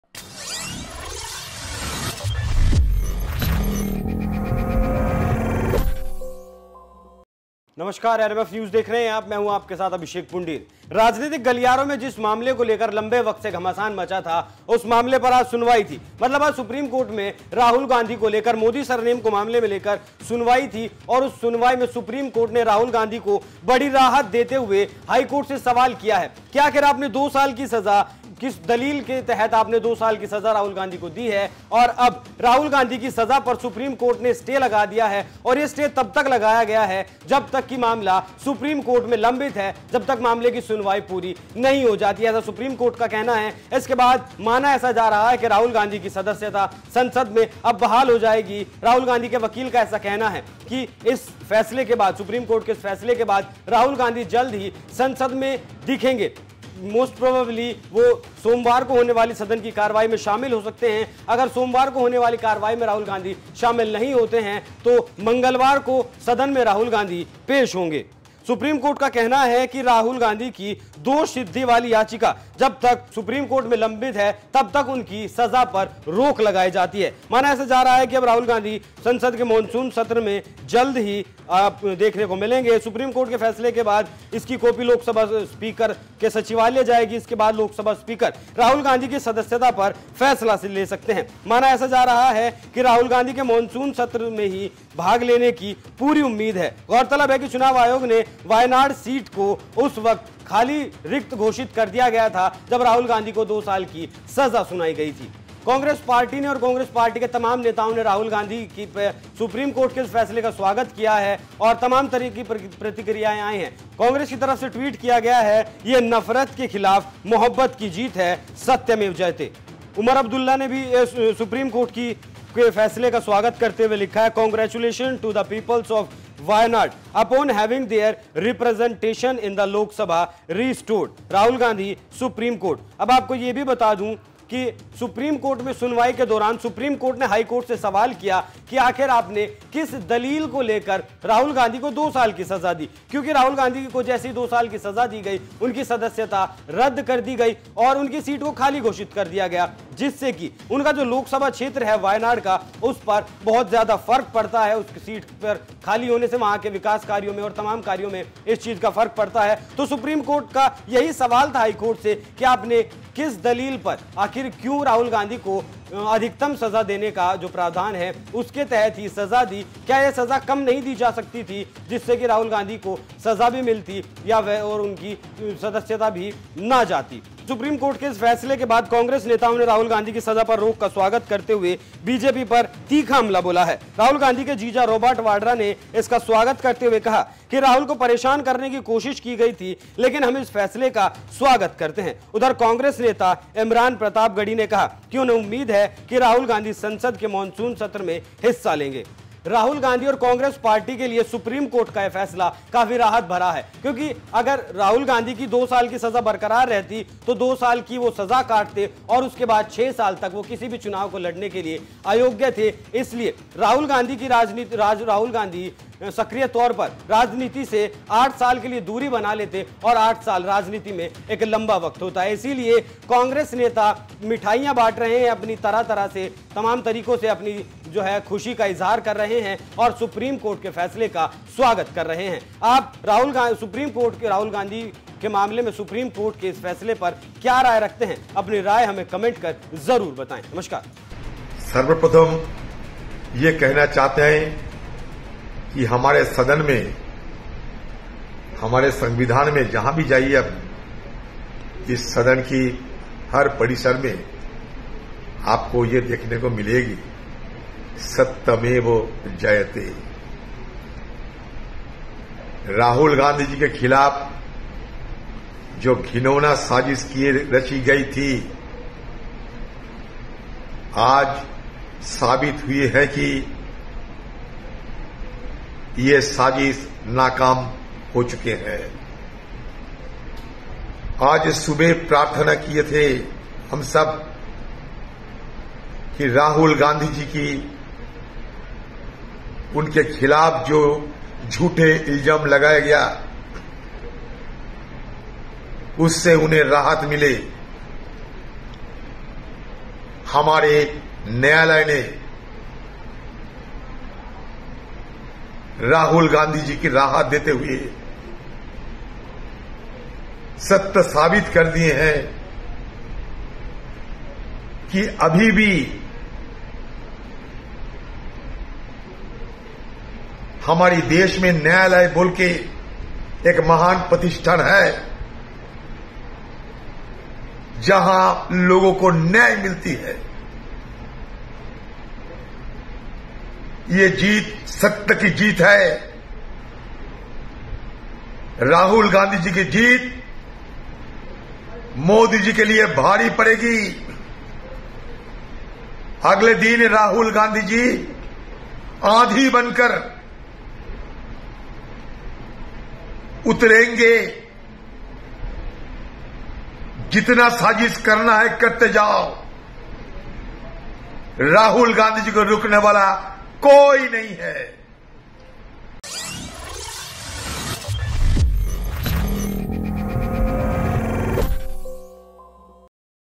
BJP को Supreme Court से झटका, जल्द की मोदी के सामने होंगे राहुल गांधी
बीजेपी को सुप्रीम कोर्ट से झटका, जल्द की मोदी के सामने होंगे राहुल गांधी

vinay
abhishek